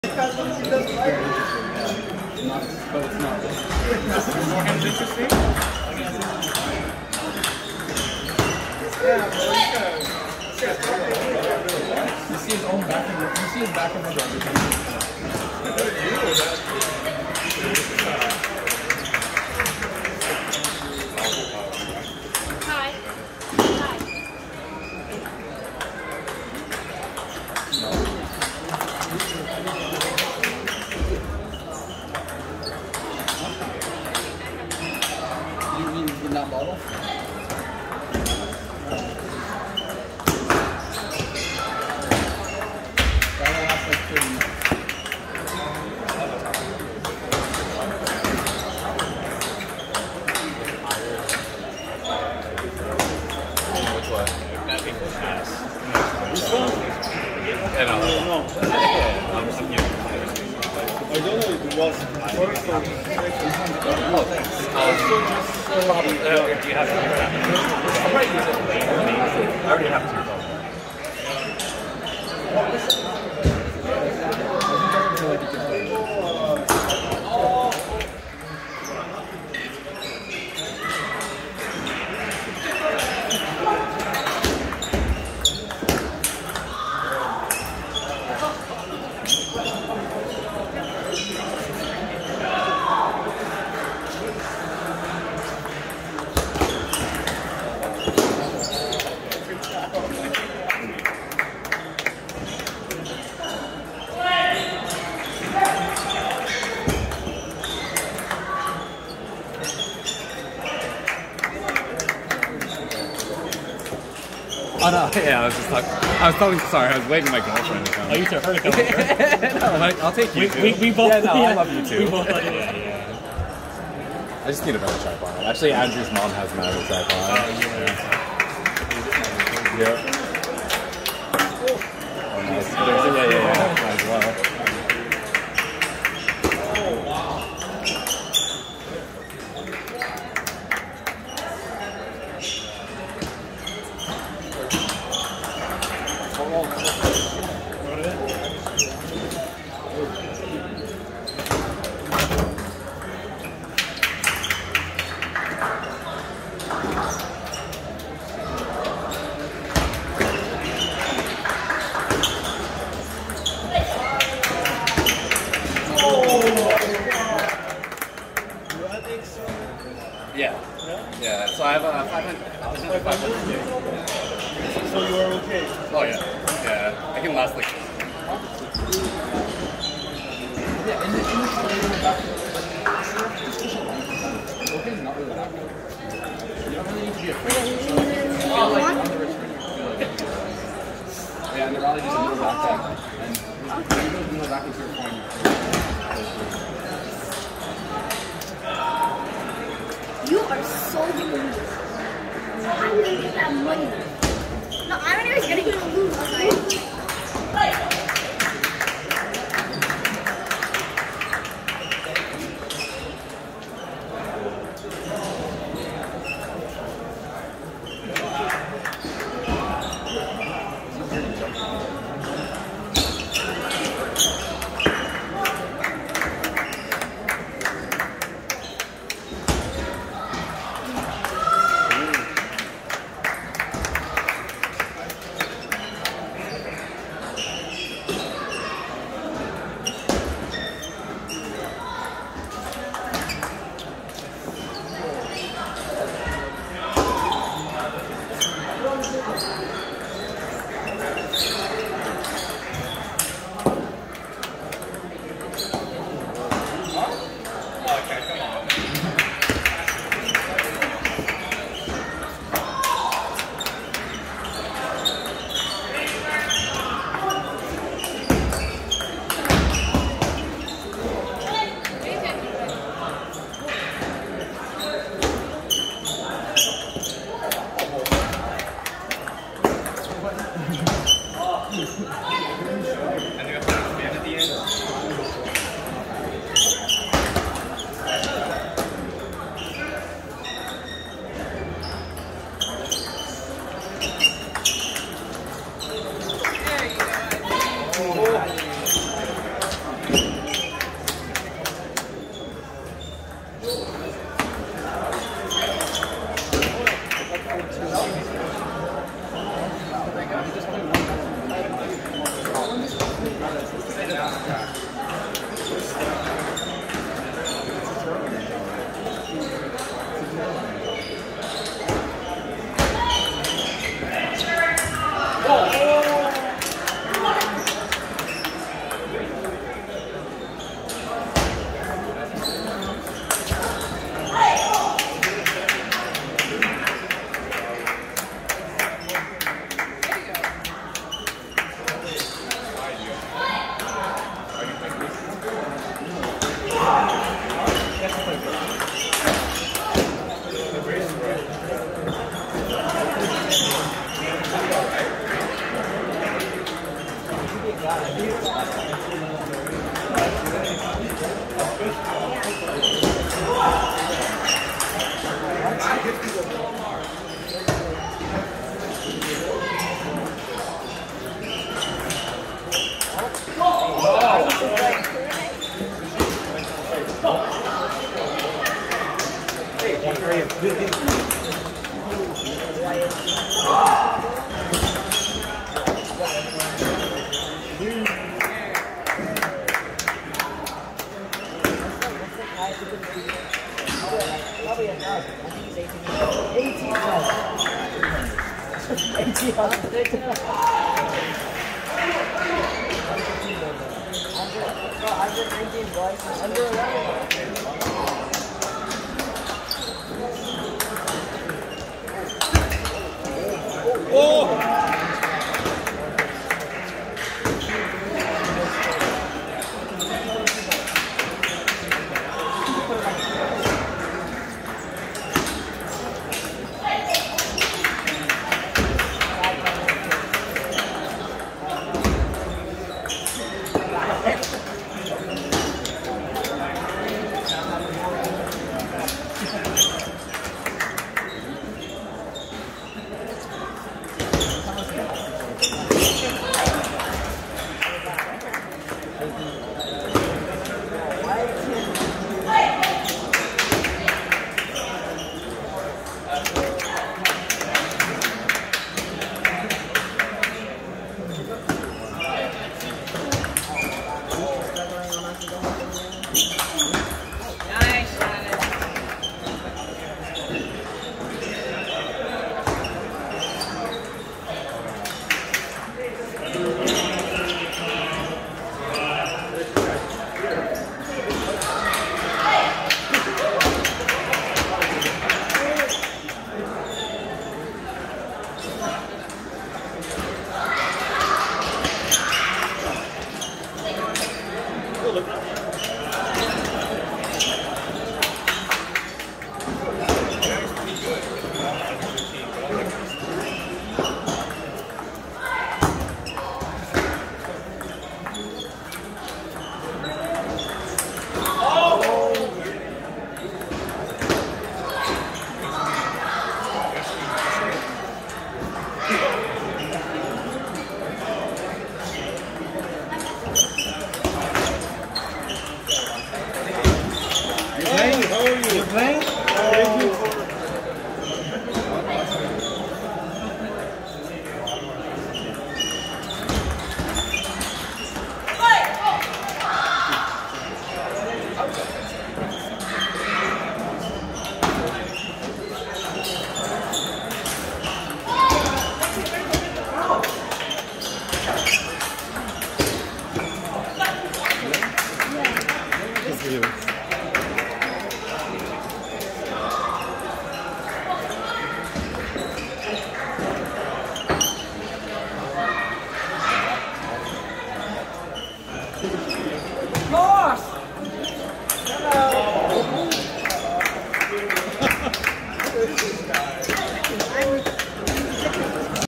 Alex Wu (2385) vs Takato Tsuchiya (2291) // 16th Si & Patty Wasserman U2550 Group - LATTA 11-13-22
In this week's tournament. Alex Wu looks to avenge the defeat of his UCLA brethren, Kai Zarehbin, to division rival Takato Tsuchiya of Cal State Northridge during last week's NCTTA tournament.

Alex is a high caliber chopper, will he be able to navigate his way around Takato's supreme feel and control of the ball?